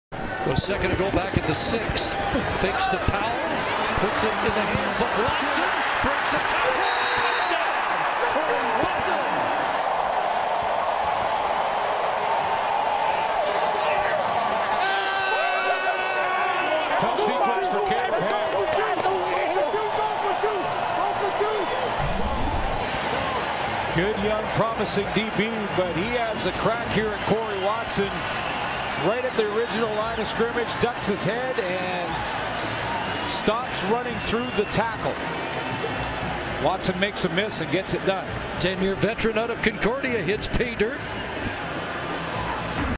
July 1, 2019 - Cody Fajardo 5 yard touchdown pass to Cory Watson
July 1, 2019 - Saskatchewan vs. Toronto